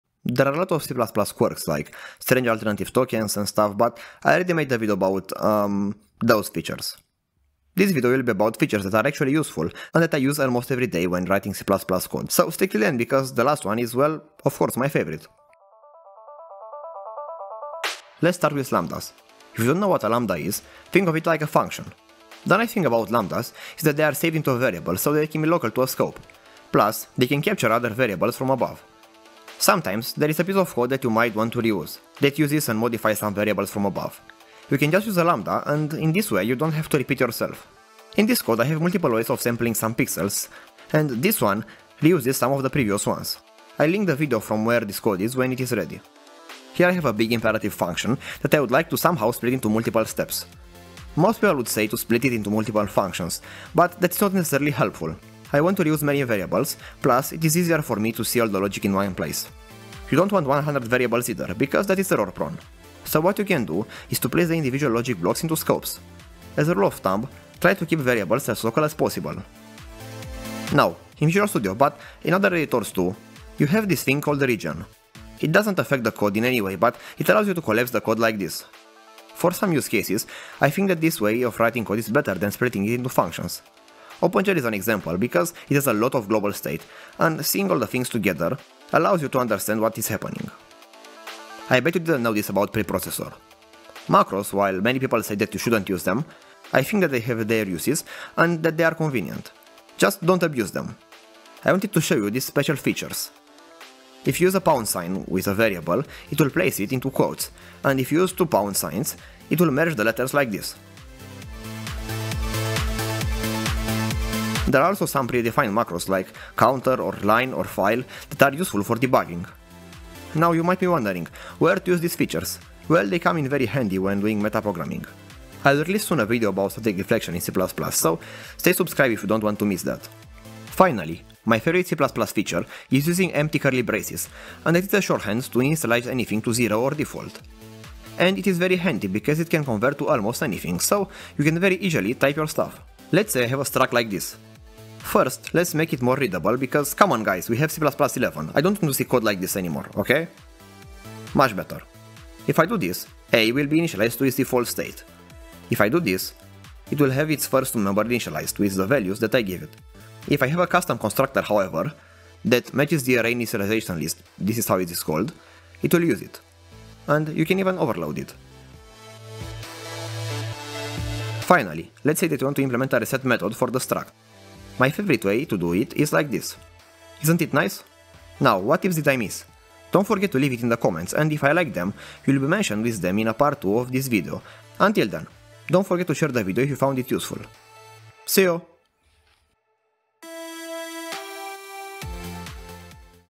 TOP 5 C++ tricks for EVERY DAY use
In this video, I will show you my favorite C++ tricks that I use almost every day!